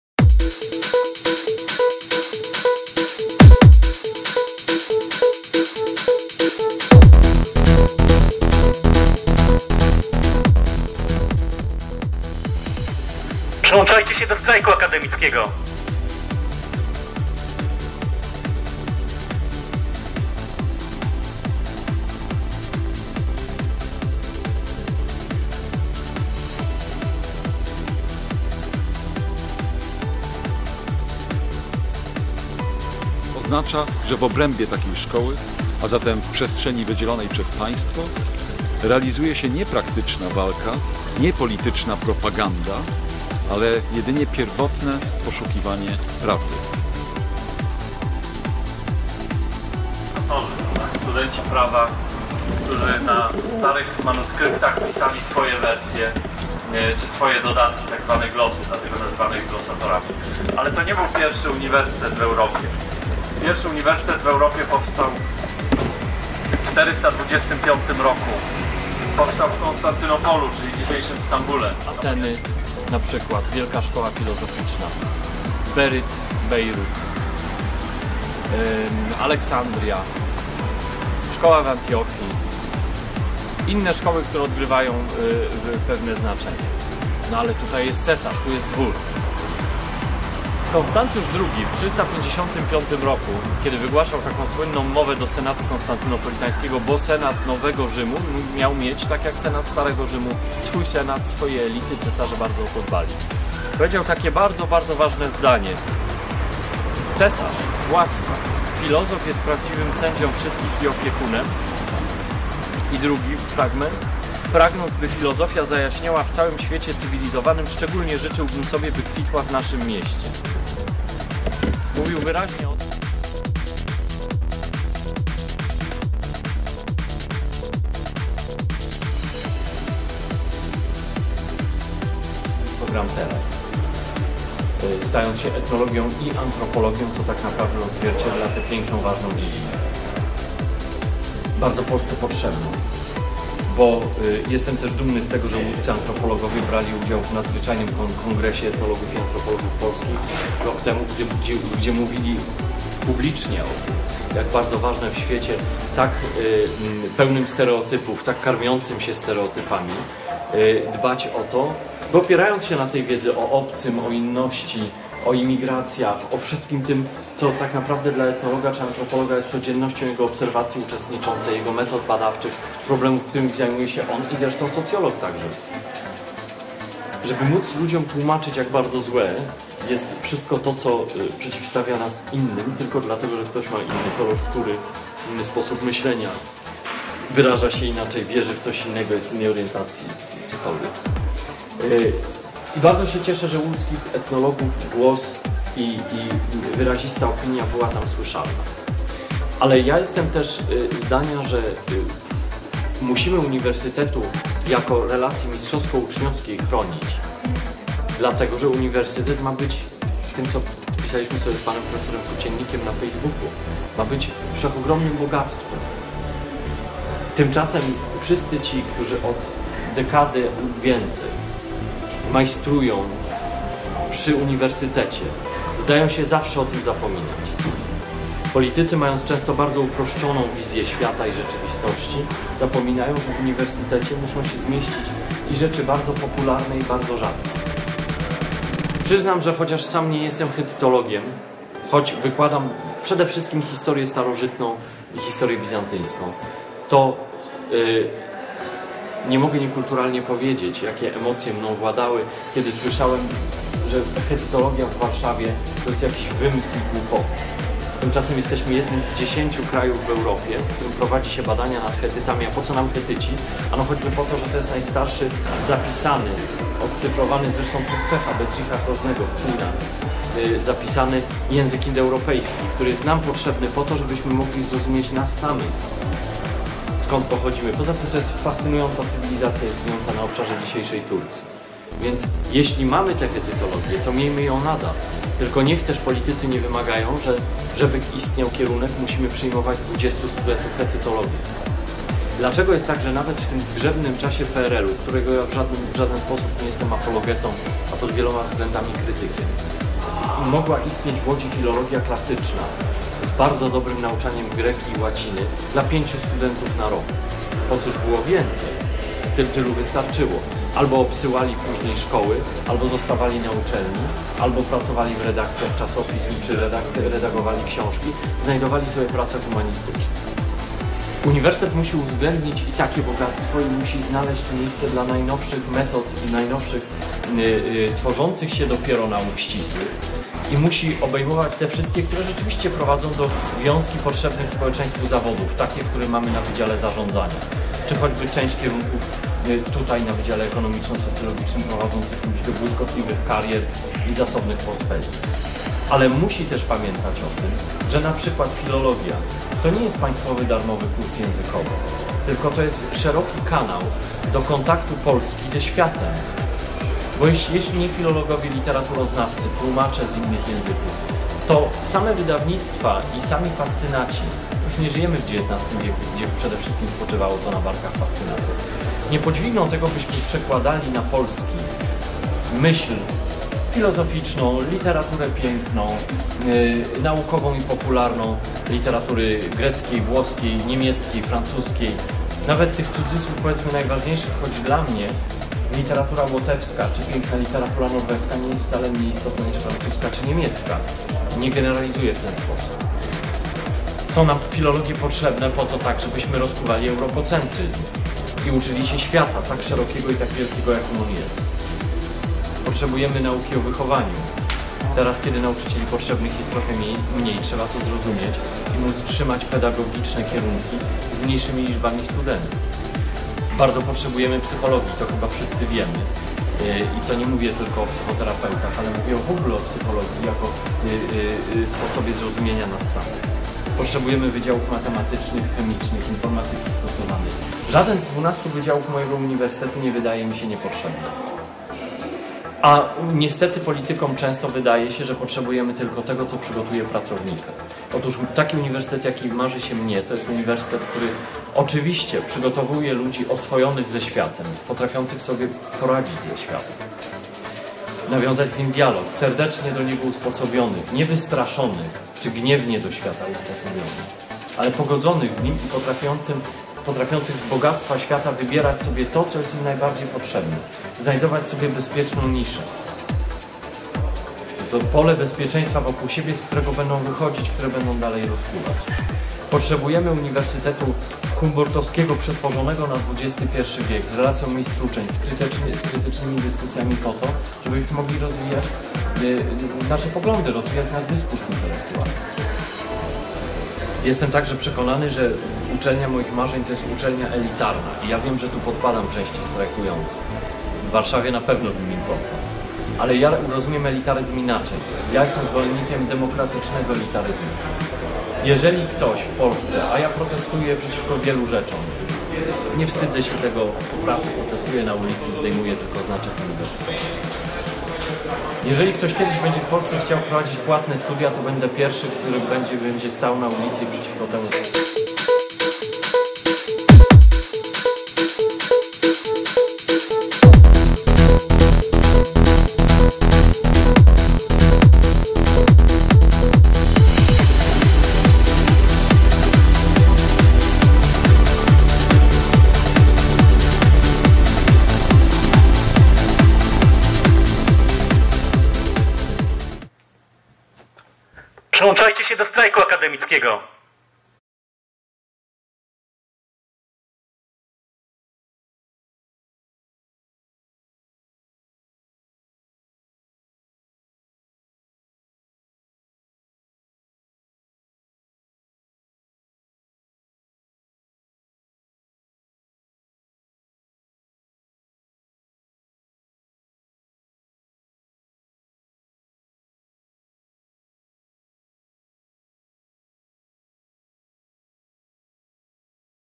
HOT15: Humanistyka Otwartym Tekstem. Wykład otwarty ze szczekaczką 1
Wykład dr Andrzeja Kompy -- wykład ze szczekaczką podczas strajku akademickiego 11 czerwca 2018 r. na EkSoc Uniwersytetu Łodzkiego